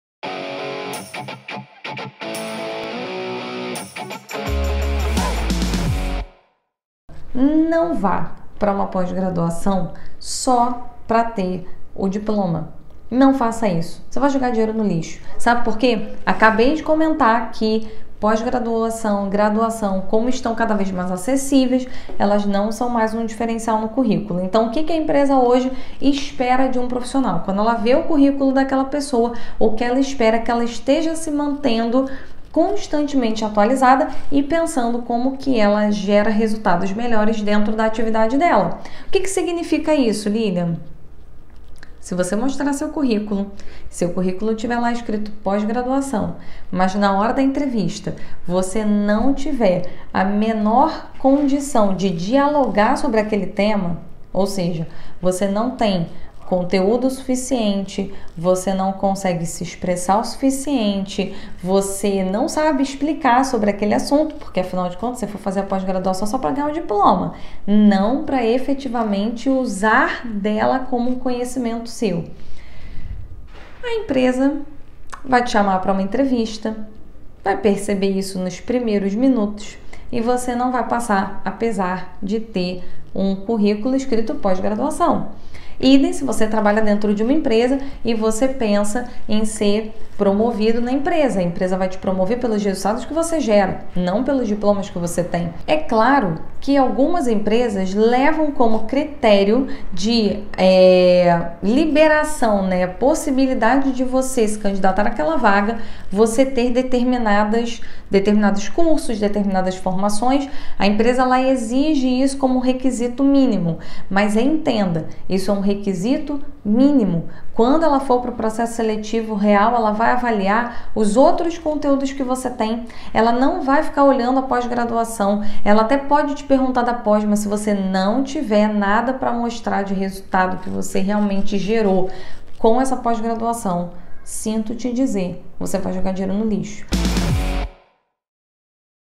Ter MBA não te promove na empresa
MBA no currículo virou commodity. O que eu quero dizer com isso? Que a grande maioria dos seus colegas de trabalho também possuem um diploma de pós-graduação. Por conta disso, as empresas não olham mais este diploma como um diferencial. O diferencial está em se manter atualizado e gerar resultados positivos onde você trabalha